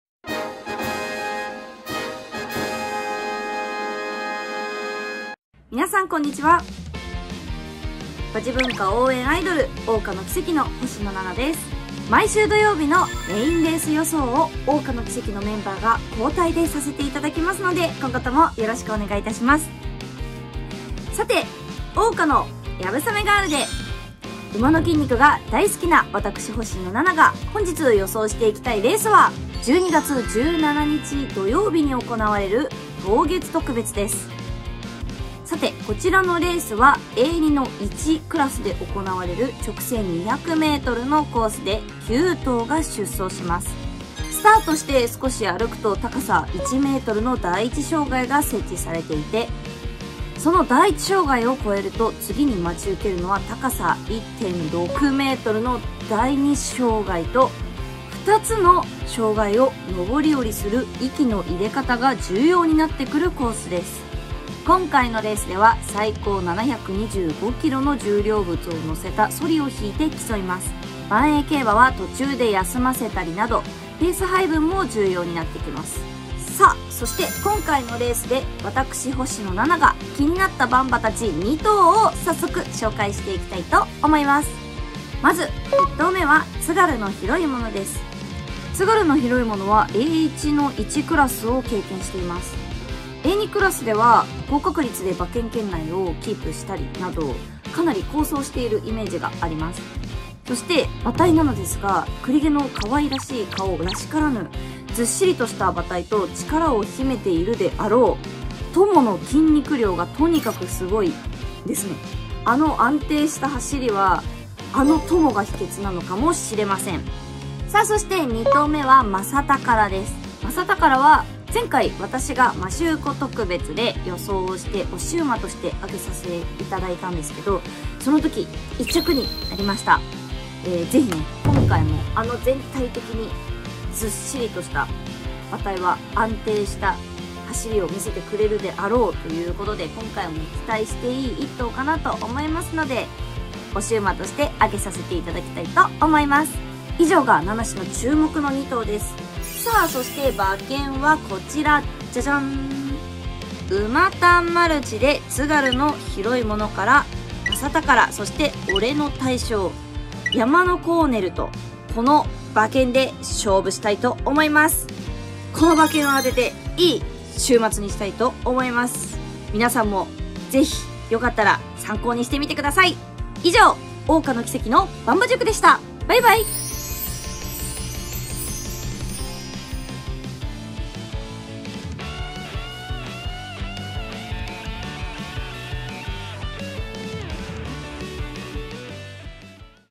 【ばんえい予想】ばんば塾～12月17日桜花のキセキ 星野菜々～
あの馬、カワイイ顔してスゴイんです！今日もななしの馬体チェックが炸裂です。

【ばんえい予想】桜花のキセキの「ばんば塾」
馬事文化応援アイドル“桜花のキセキ”星野菜々が予想します。

■出演者
・桜花のキセキ